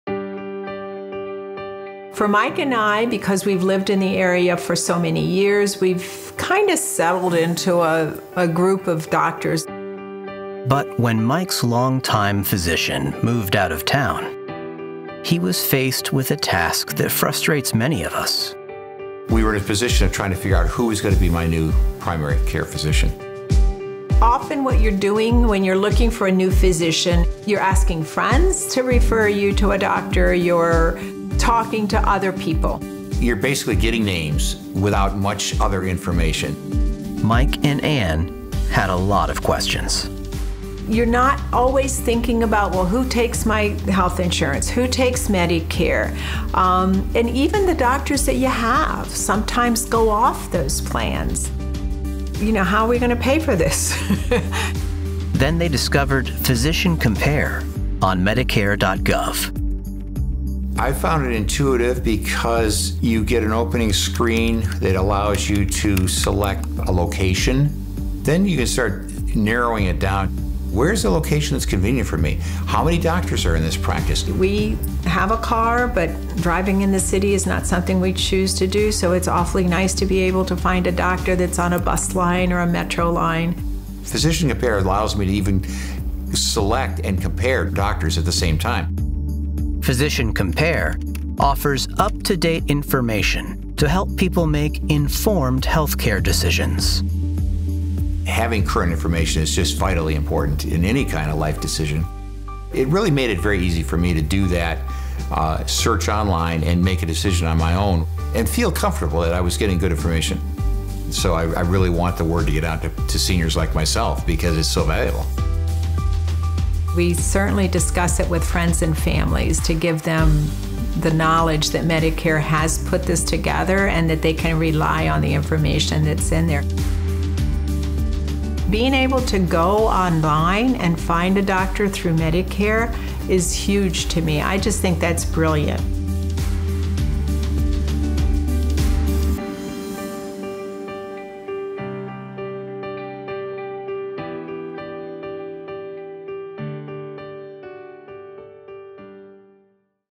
CMS Physician Compare Patient Story
This video from the Centers for Medicare & Medicaid Services (CMS) features the true story of two Medicare beneficiaries, Mike and his wife Ann of Washington, D.C. The couple needed help finding a clinician in their area but didn’t know where to turn. Watch as they explain the reasons they consider Physician Compare to be such a valuable search tool and why they believe their friends and others should use it, too.